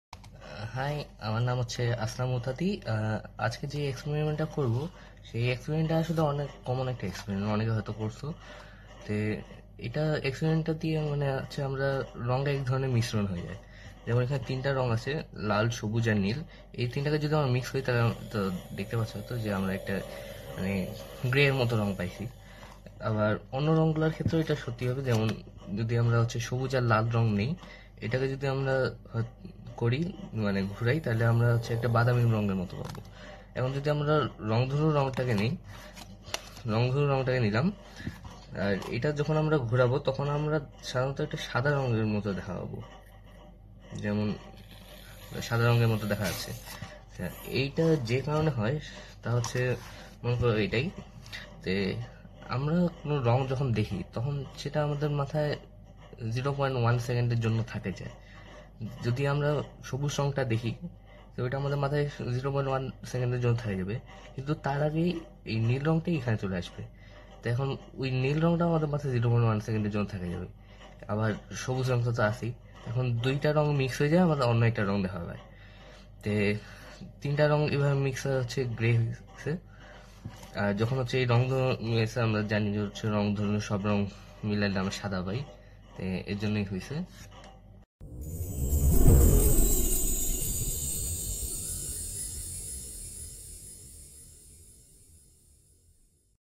Color Mixing | আলো নিয়ে সায়েন্স এক্সপেরিমেন্ট | IDL 2020
শিরোনাম: Colour mixing

এক্সপেরিমেন্টকারি: আসনাফ মুহতাদি
প্রতিষ্ঠান: Dinajpur zilla school , Dinajpur

আইডি: IDLE20032

আন্তর্জাতিক আলোক দিবস ২০২০ উপলক্ষ্যে নানা আয়োজনের অংশ হিসেবে বাংলাদেশ বিজ্ঞান জনপ্রিয়করণ সমিতি আয়োজন করছে এই আলোকচিত্র প্রতিযোগিতা, 'আলো নিয়ে সায়েন্স এক্সপেরিমেন্ট'।